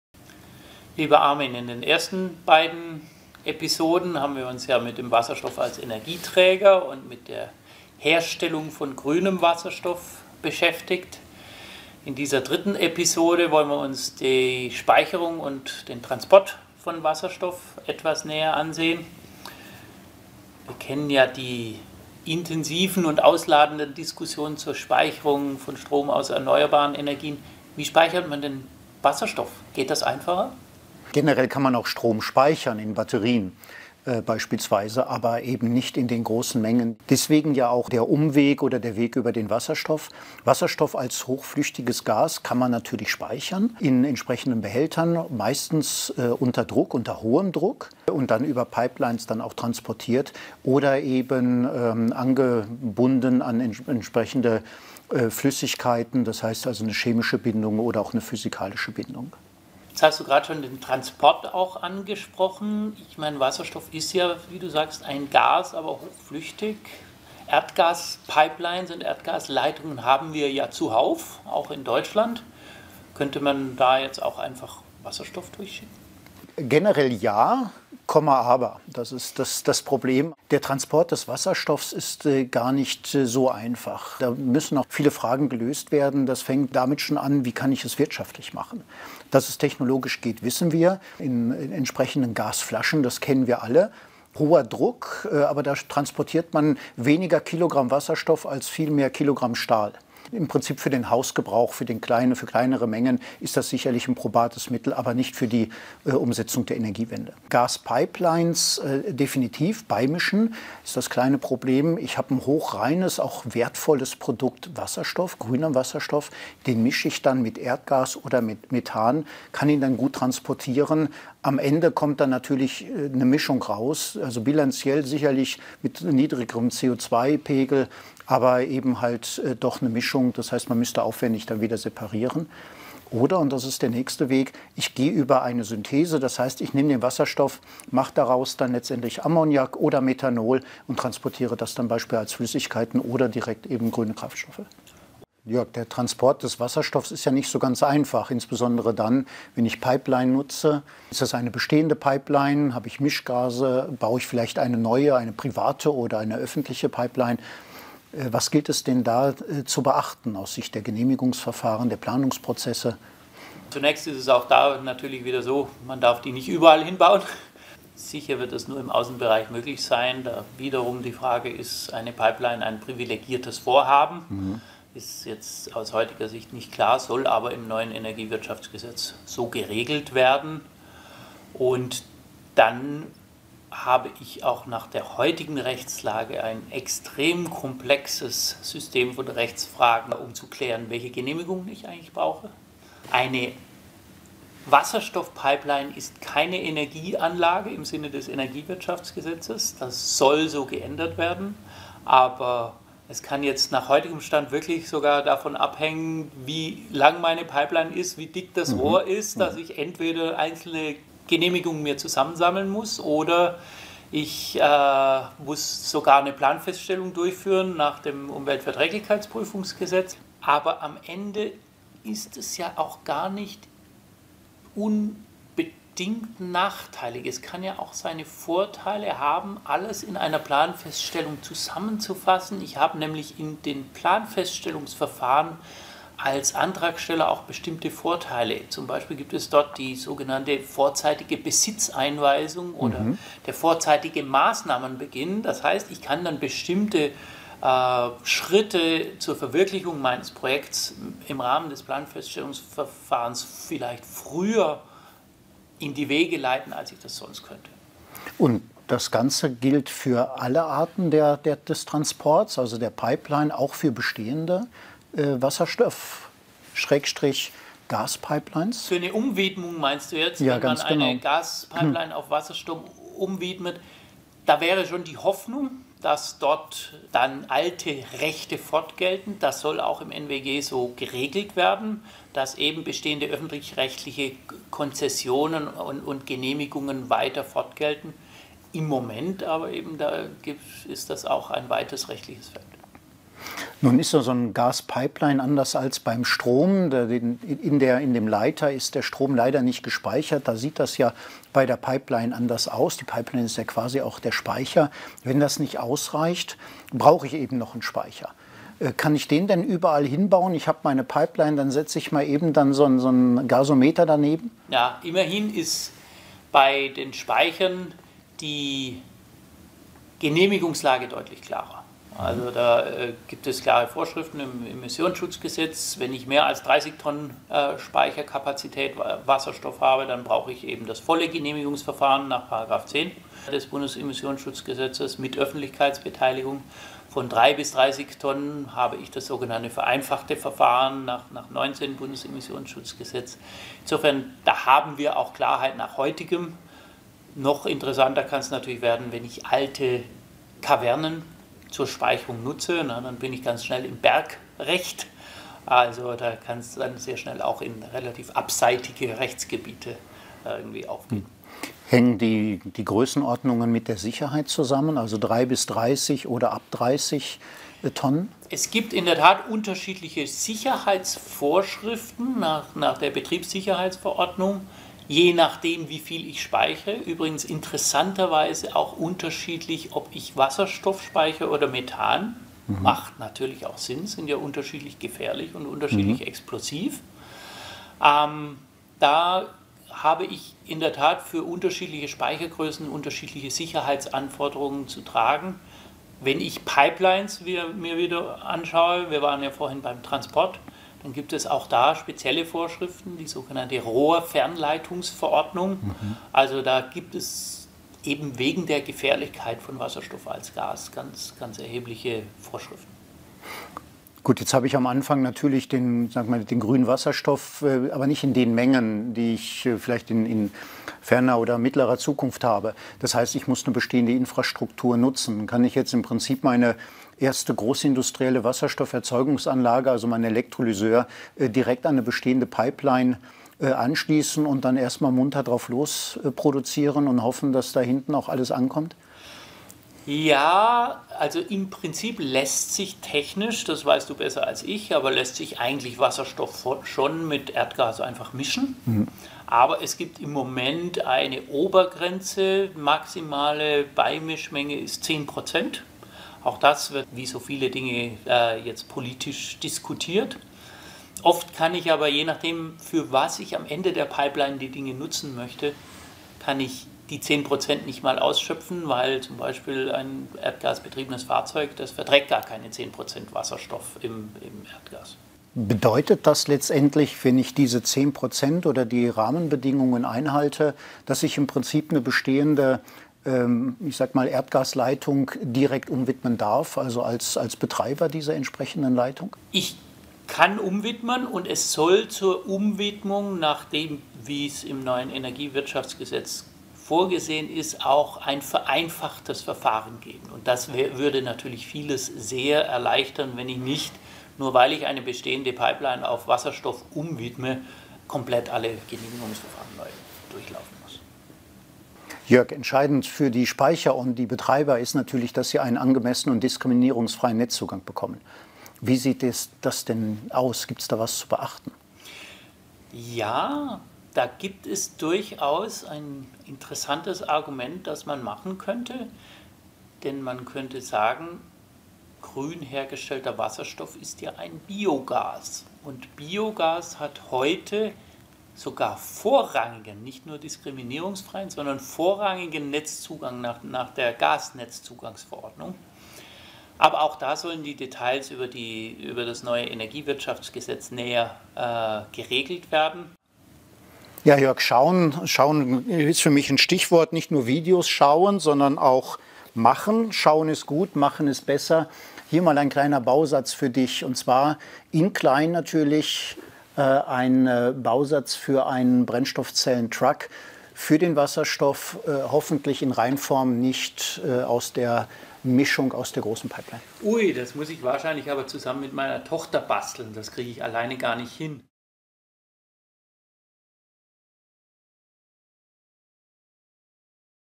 Transport und Speicherung von Wasserstoff
In Episode Nummer 3 unseres H2-Videopodcast geht es um den Transport und die Speicherung von Wasserstoff. Unsere beiden Experten Dr. Jörg Häring, Mitglied des Executive Committees und Chief Legal and Assurance Officer bei CEPSA, und Prof. Dr. Armin Schnettler, Siemens Energy und Präsident des VDE, diskutieren dazu die tatsächlichen und rechtlichen Fragestellungen.